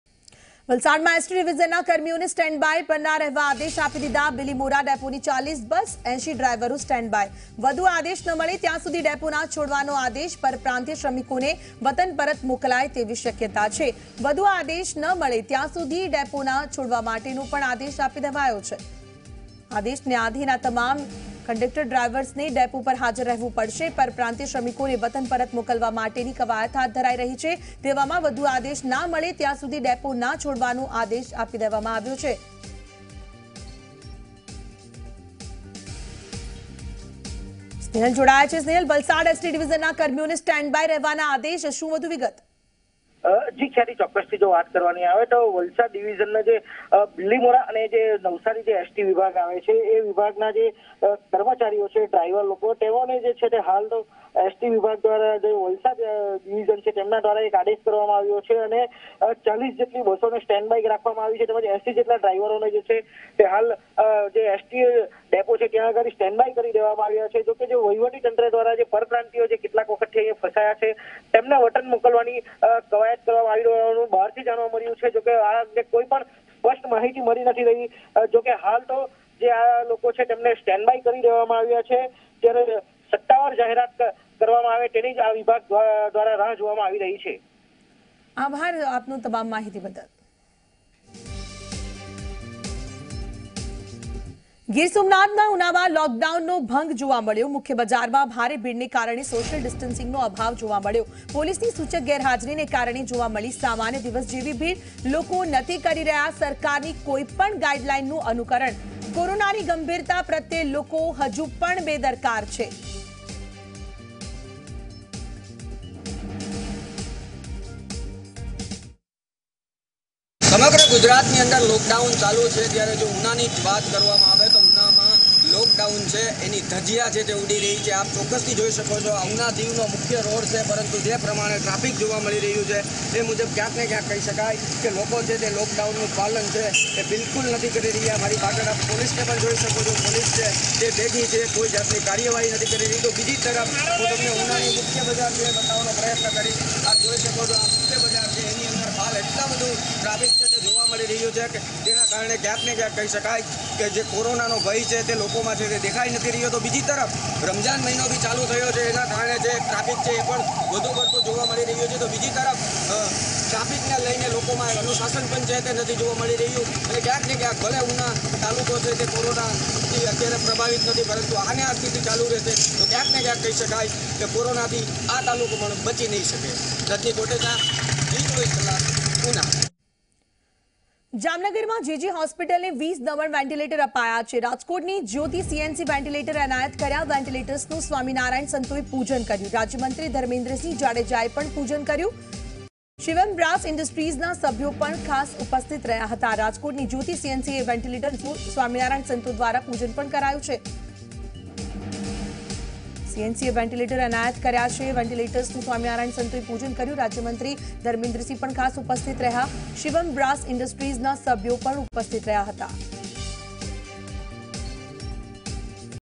Valsad | વલસાડ | ST Department | એસટી વિભાગ | Employee | કર્મચારી | Gujarati News On Zee
વલસાડના ST ડિવિઝનના કર્મીઓને સ્ટેન બાય પરના આદેશ

વલસાડ એસટી ડિવિઝને પોતાના કર્મચારીઓને સ્ટેન્ડ બાય રહેવાનો આદેશ કર્યો છે. આ સાથે નવસારી એસટી વિભાગના કર્મચારીઓને સ્ટેન્ડ બાય રહેવાનો આદેશ કરાયો છે. નવસારી અને બીલીમોરા એસટી ડેપોના 40થી વધુ બસો સાથે 80 જેટલા ડ્રાઇવરોને ડેપો પર સ્ટેન્ડ બાય રાખ્યા છે. વિભાગને આદેશ ન મળે ત્યાં સુધી બસ ડેપો ન છોડવાની સૂચના અપાઈ છે.